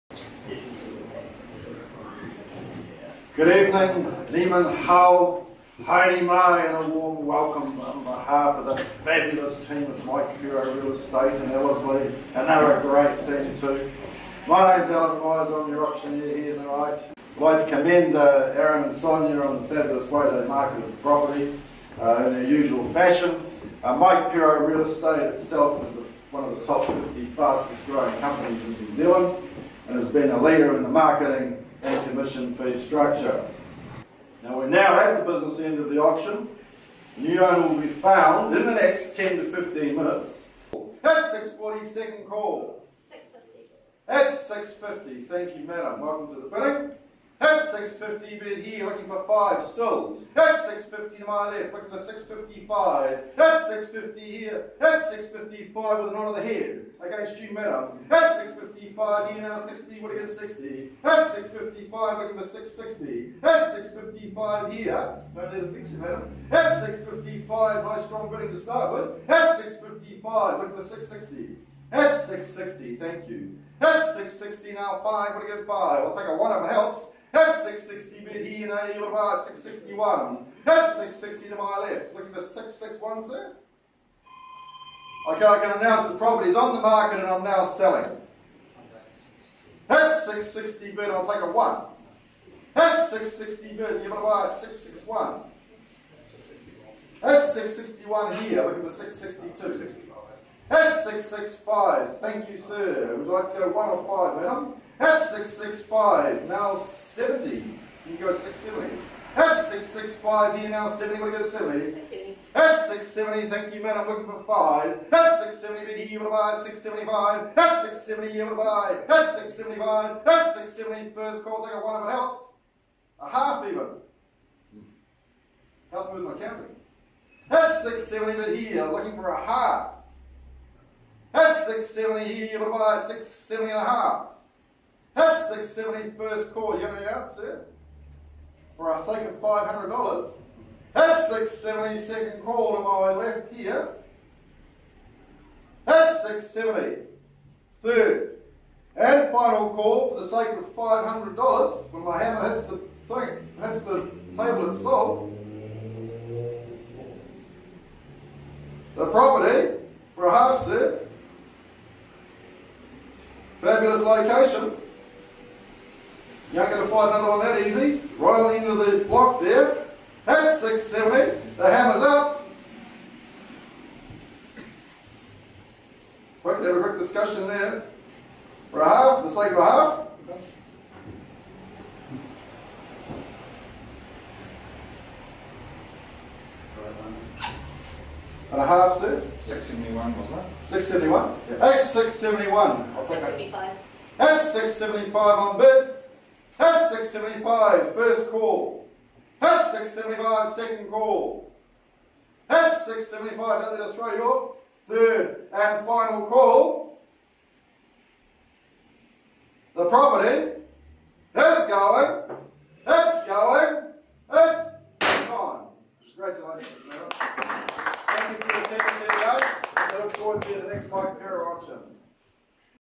Sold Under the Hammer-30.2 Cawley St, Ellerslie
Another successful auction campaign by Aaron & Sonya Telfar of Mike Pero Real Estate, Ellerslie Office, Auckland